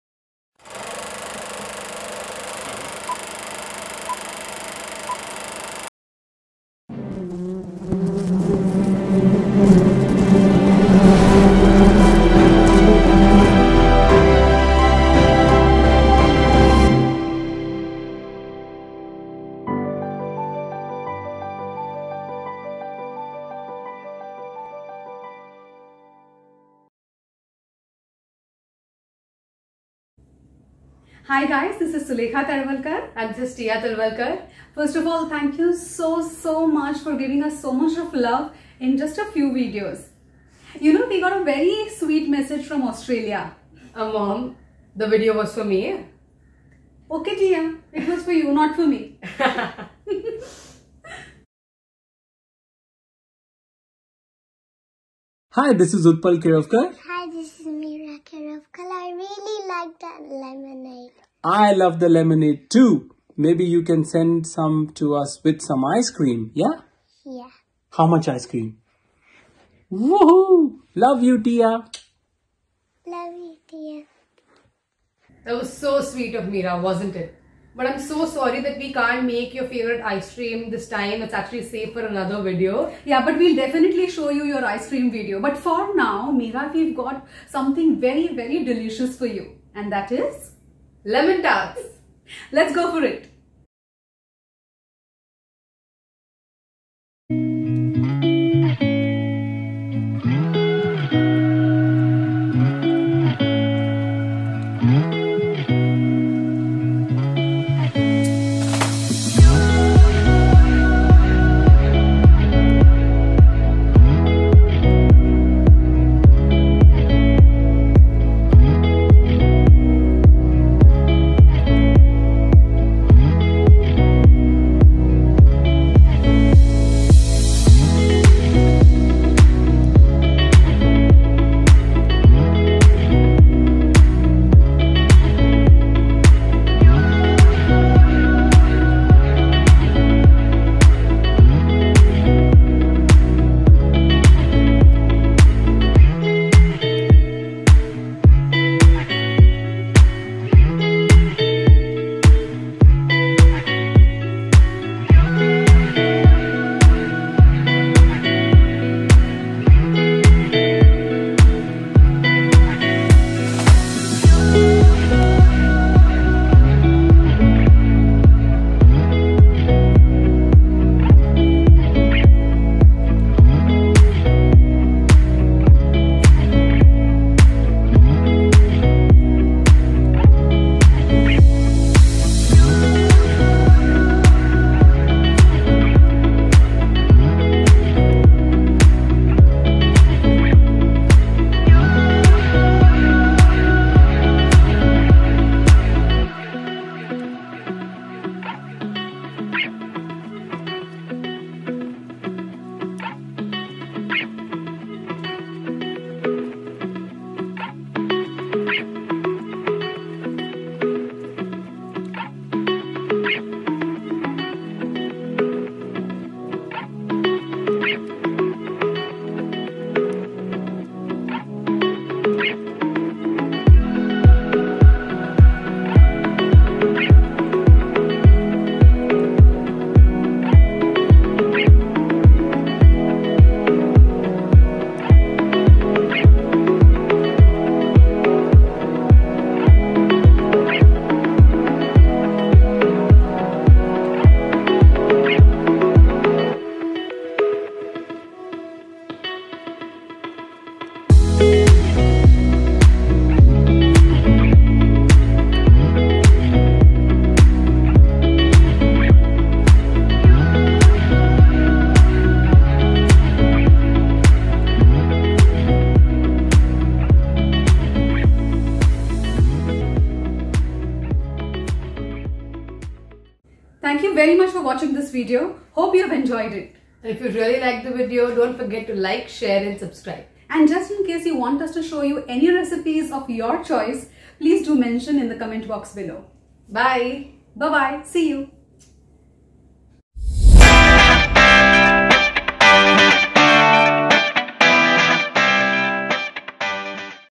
Delicious Lemon Tart
Mouth Watering easy Lemon Tarts made from scratch.

Making Lemon Tarts is Childs Play . The Recipe is Lengthy , but easy ,yet Delicate.

Recipe by
Tia Talwalkar

Concept:
Sulekha Talwalkar & Maneesha Soman

Camera:
Arya Talwalkar

Technical support:
Akshay Bapat